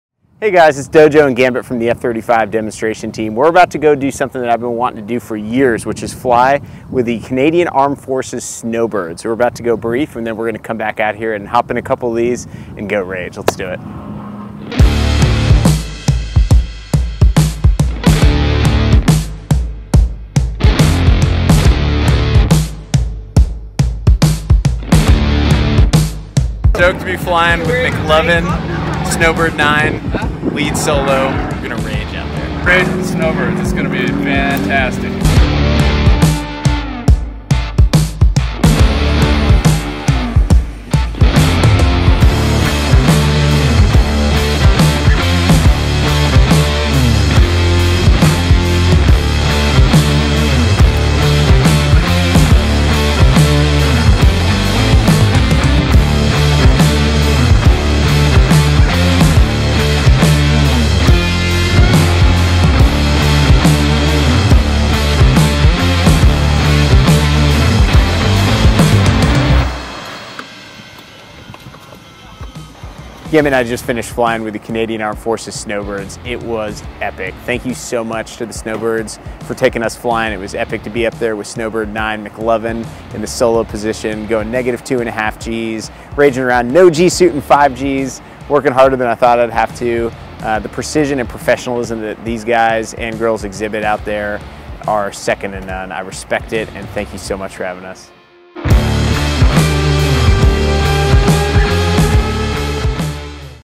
F 35 Demo Team pilot rides shotgun with the RCAF Snowbirds
F-35 Demo Team pilot and commander, Capt. Andrew "Dojo" Olson and his wingman Maj. Aaron "Gambit" Stevens, went along for a ride with the Royal Canadian Armed Forces Snowbirds during their practice show at the 2019 Aero Gatineau-Ottawa Airshow. (U.S. Air Force Video by Senior Airman Alexander Cook)